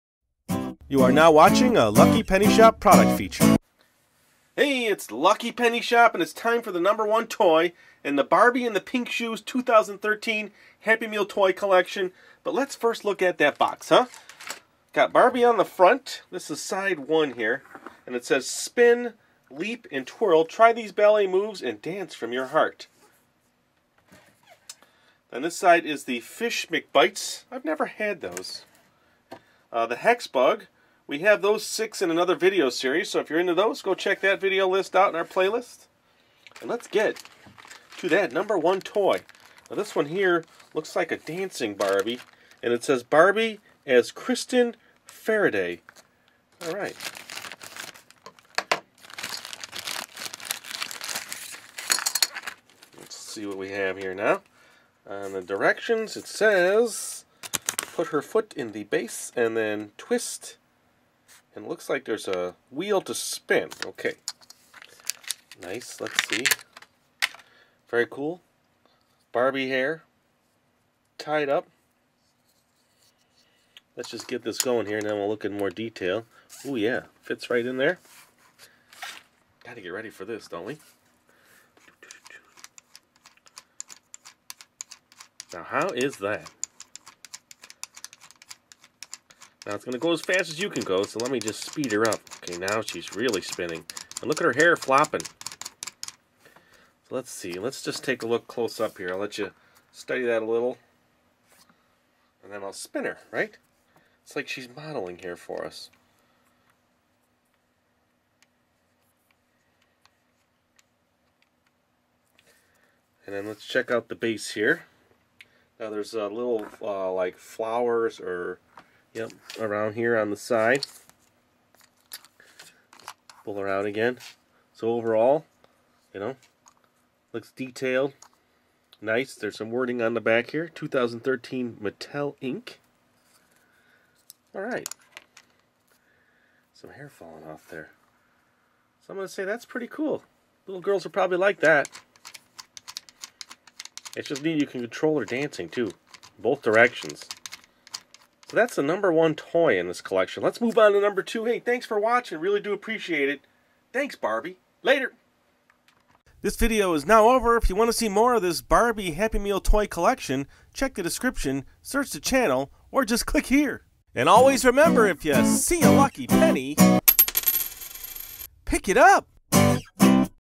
Barbie in The Pink Shoes #1 Barbie as Kristyn Farraday - 2013 McDonald's Happy Meal Toy Review
Watch our product feature video for Barbie in The Pink Shoes #1 Barbie as Kristyn Farraday - 2013 McDonald's Happy Meal Toy Review.